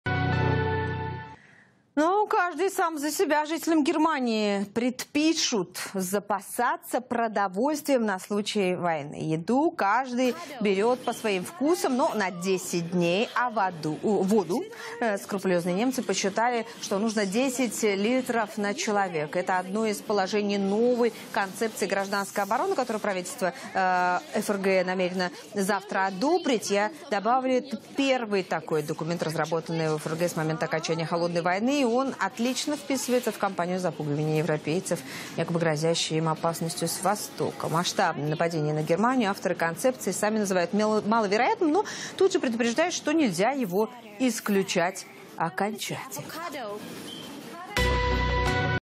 Жителей Германии обяжут запасаться едой на случай войны
В Германии приняли новую концепцию гражданской обороны